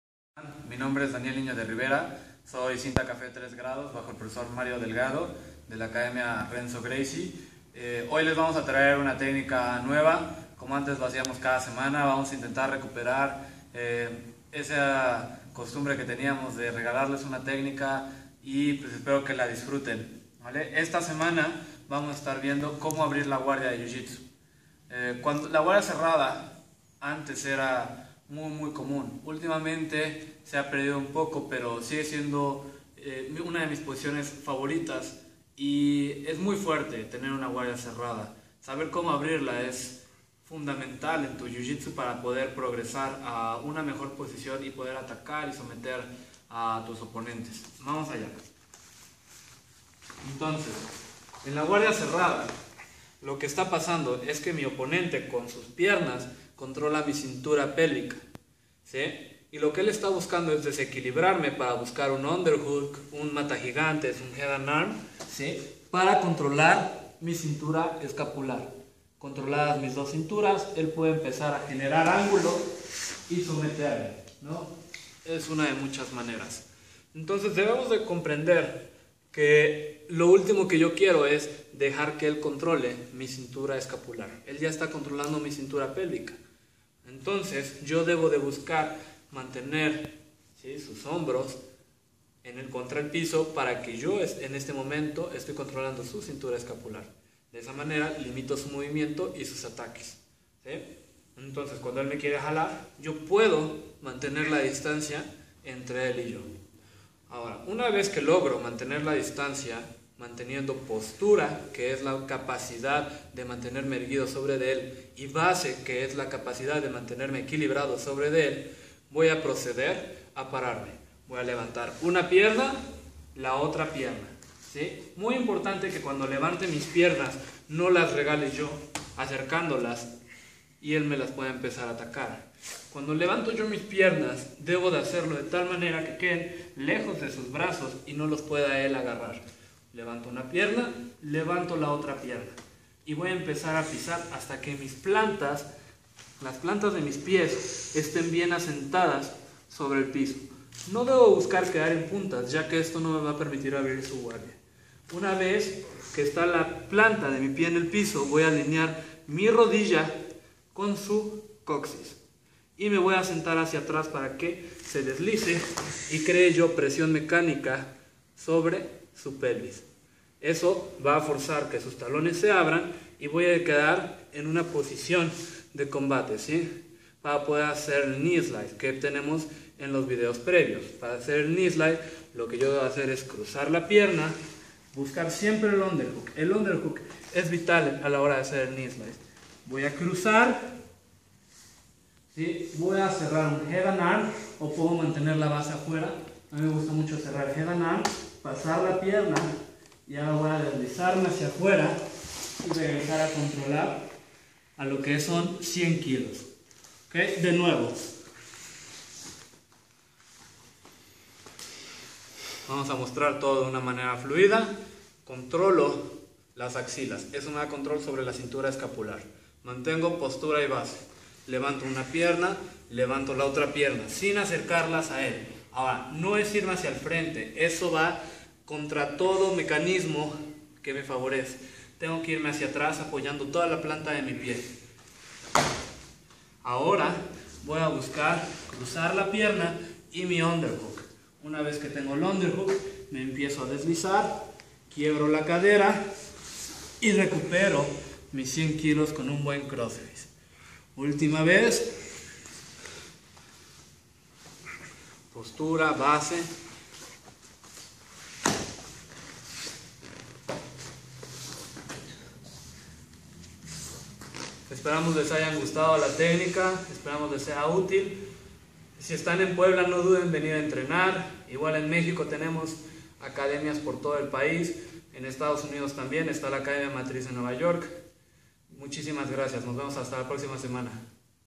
Cómo abrir la guardia cerrada de jiu jitsu
Un video tutorial con los detalles de como abrir la guardia cerrada de jiu jitsu brasileño explicada por nuestro profesor Daniel Niño de Rivera,  cinturón café 3 grados bajo Mario Delgado, nuestra academia en puebla se encuentra en la 35 poniente 3111 esquina 33 sur colonia el vergel, visita nuestra página de facebook Renzo Gracie Puebla